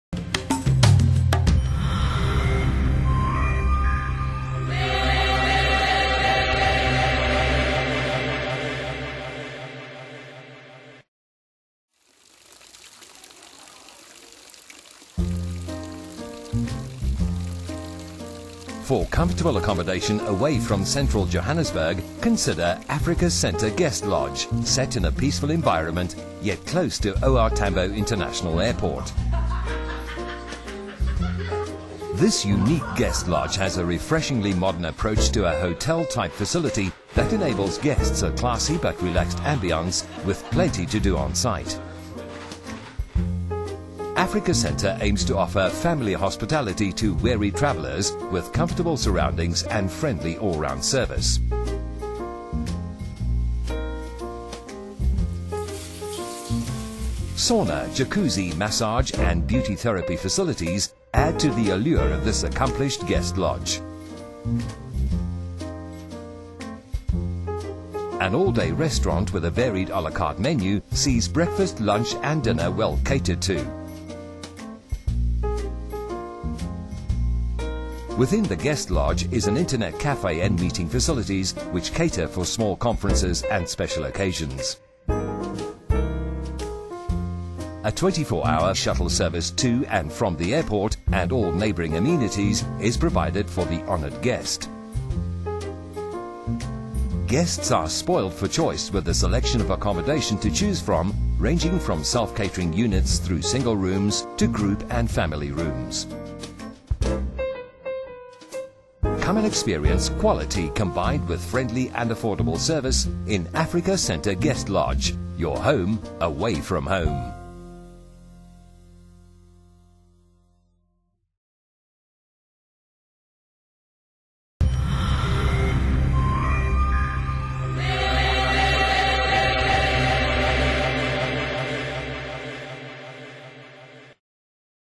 Africa Centre Guest Lodge Benoni - Africa Travel Channel
Africa Centre Guest Lodge - For comfortable accommodation away from Central Johannesburg, consider Africa Centre Guest Lodge, set in a peaceful environment, yet close to OR Tambo International airport. This unique guest lodge has a refreshingly modern approach to a hotel-type facility that enables guests a classy but relaxed ambiance with plenty to do on-site. Africa Centre aims to offer family hospitality to weary travellers with comfortable surroundings and friendly all round service. Sauna, Jacuzzi, massage and beauty therapy facilities add to the allure of this accomplished guest lodge.